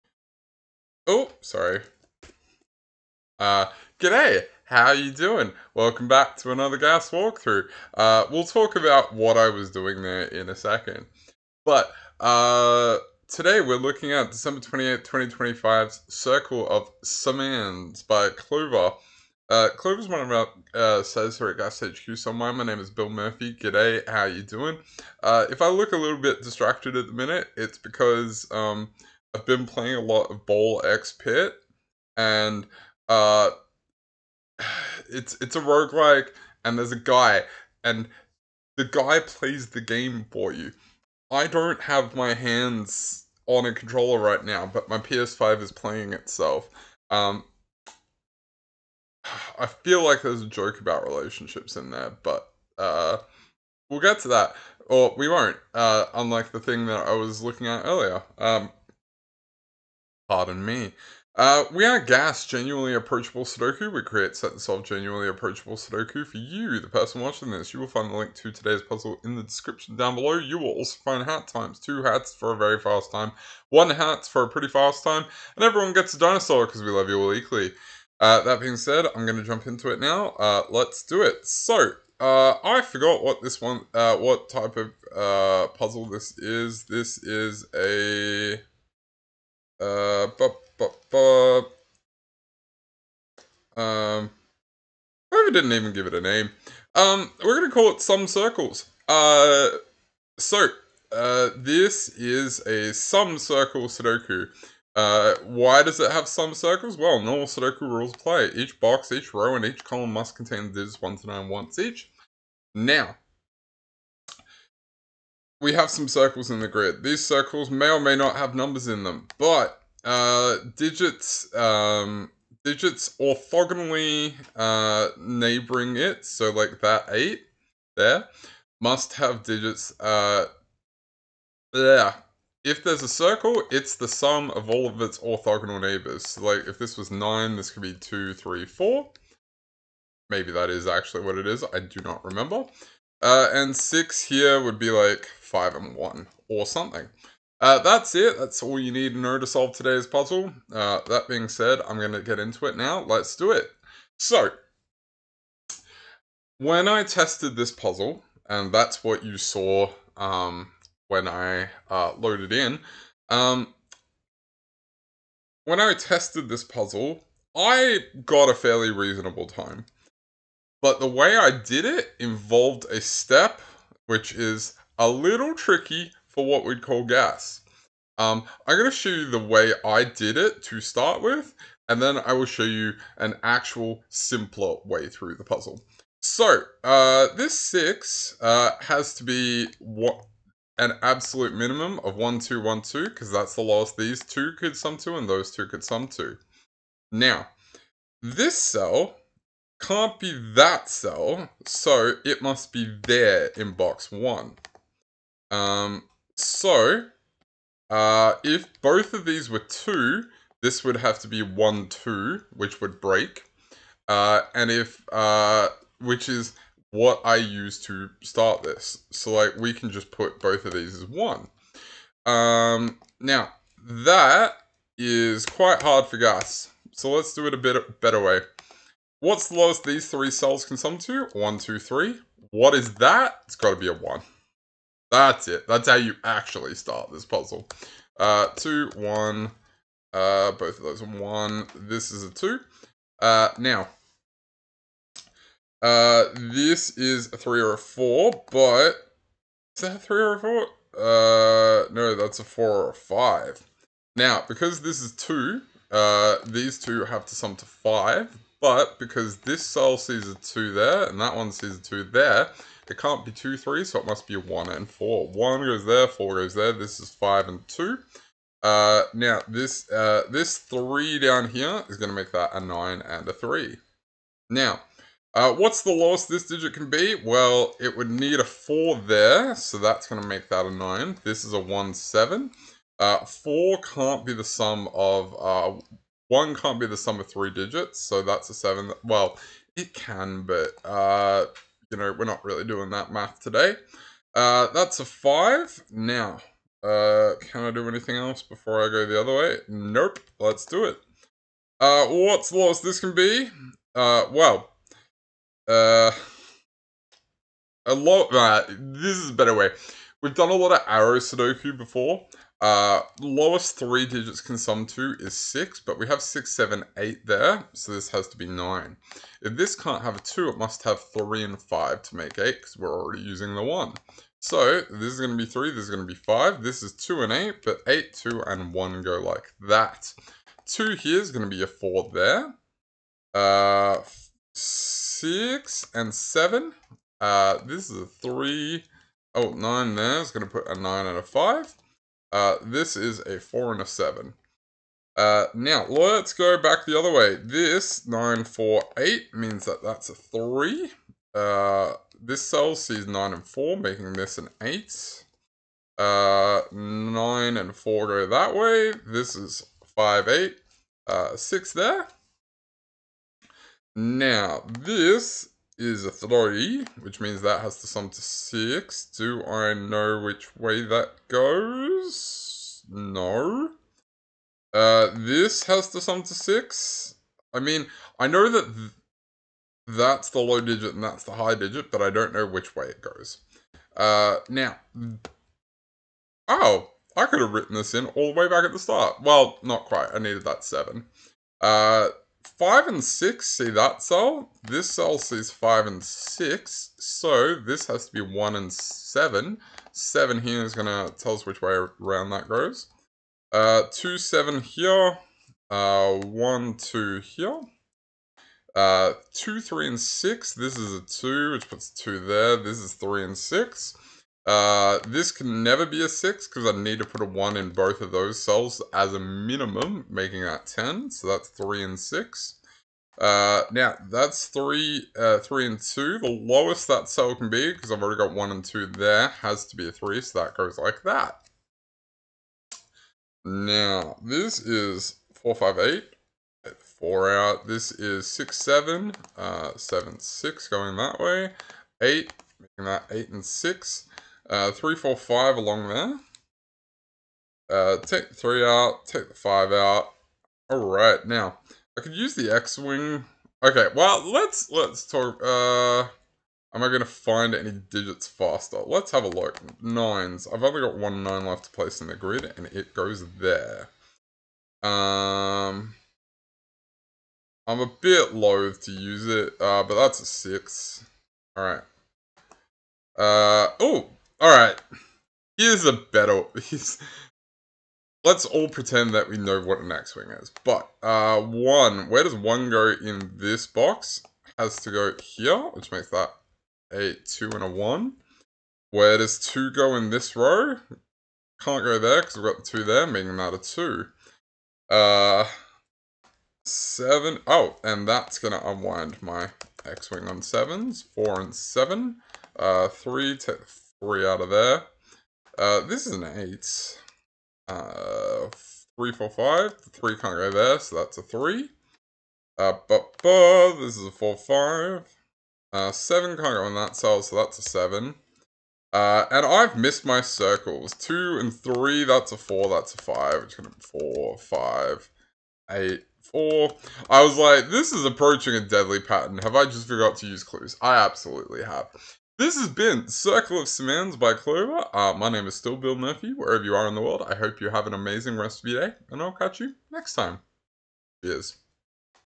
GAS Sudoku Walkthrough - Circle of Summands by Clover (2025-12-28)
Bill solves Clover's Circle of Summands, originally posted 28th of December, 2025.

Normal sudoku rules apply. A circled number in a cell must equal the sum of all orthogonally adjacent numbers to that cell.

Turns out it's called "Sum Adjacent", I just lost my mind.

For two party hats, finish within 6:30. For one party hat, finish within 12:00. All other solvers will earn a Rubenesque Rubeosaurus. 🦕